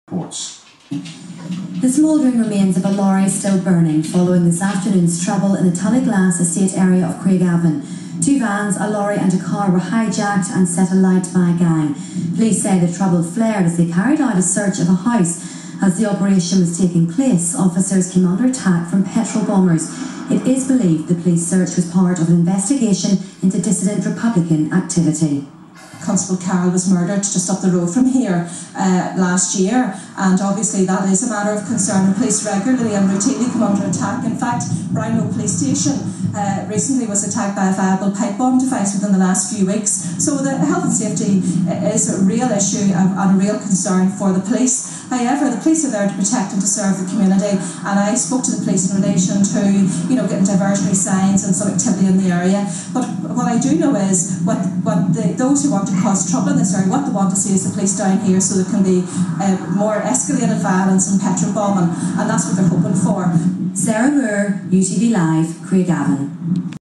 Republican Gangs Riot In Craigavon
Irish Republican gangs out wrecking the community once again.  Most no doubt , not even knowing why they are doing it , but doing it simply because they are told to or because they are the local hoods & thugs who enjoy wrecking & torturing people. The British Northern Irish Protestants & Catholics of Northern Ireland want to live in peace. These republican gangsters only want money , they know that there is no chance of a 32 county Ireland. Infact they no there is more chance of a United States Of The World.